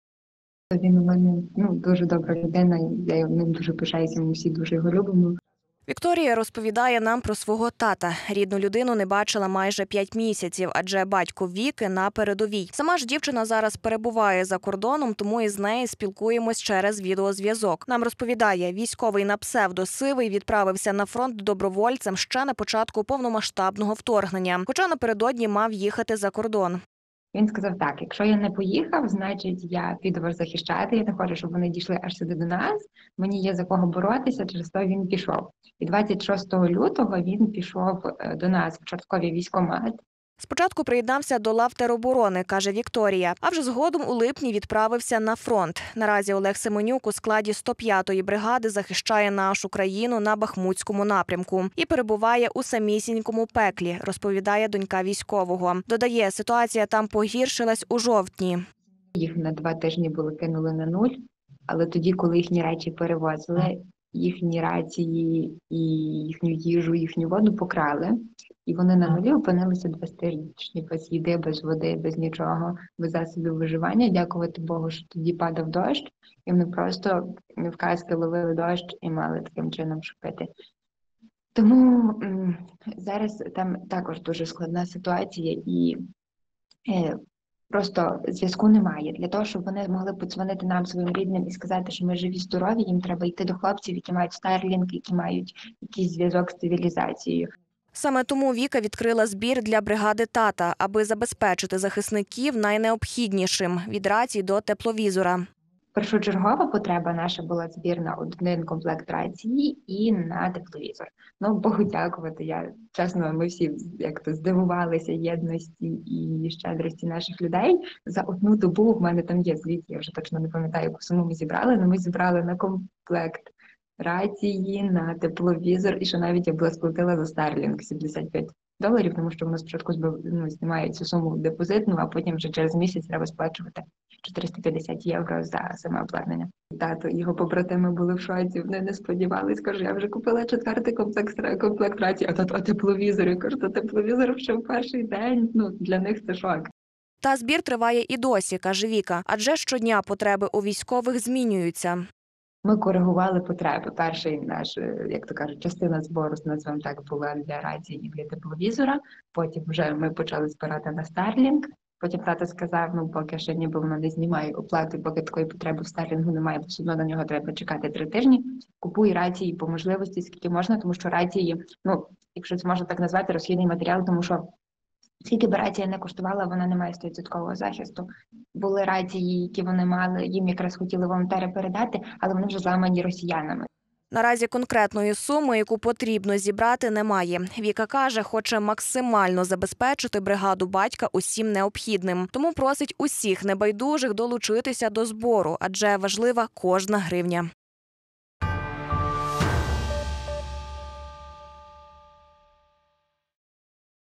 Донька військового з Тернопільщини оголосила збір для бригади тата
Продовжуємо працювати на перемогу України разом. Олег Семенюк із позивним "Сивий" захищає нашу країну із початку повномасштабного вторгнення. Тепер сам із побратимами потребує допомоги. Дочка військового оголосила збір. Детальніше — у сюжеті.